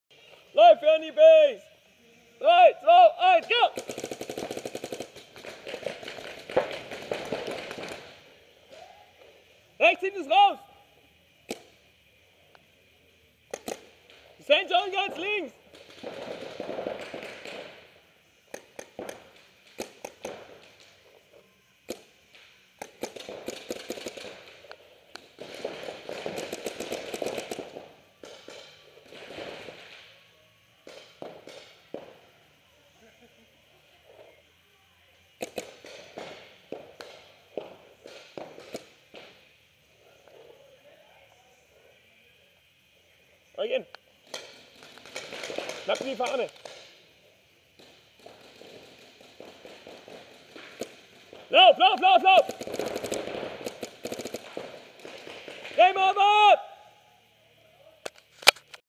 Paintball - Schnell und Geil No.223 - ganzes Team auf der Mittellinie + Eugen als Fahnenträger
Paintballspielen in Stuttgart-Stammheim am 17.05.12
Kamera: Contour HD eingestellt auf 720p mit 60fps